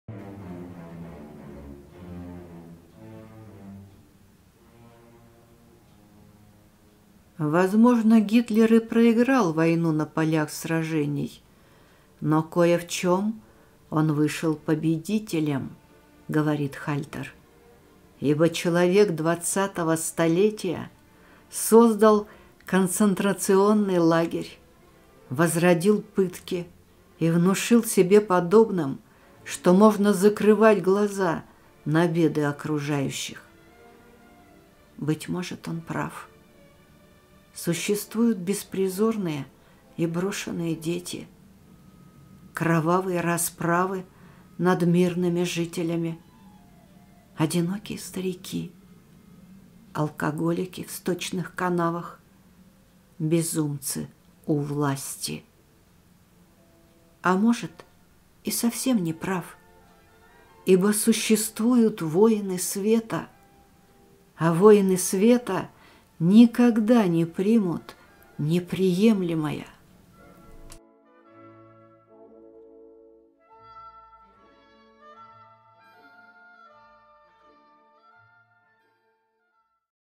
П Коэльо О неприемлемом Притча 70 Книга воина света
Притча (70) из книги бразильского прозаика и поэта П.Коэльо "Книга воина света". О неприемлемом.
Перевод с португальского А.Богдановского.
Фотография Е.Шавриной. Пермский край.
Классическая музыка   из фонотеки Ютуб Classical Music
Загрузил: Konstantin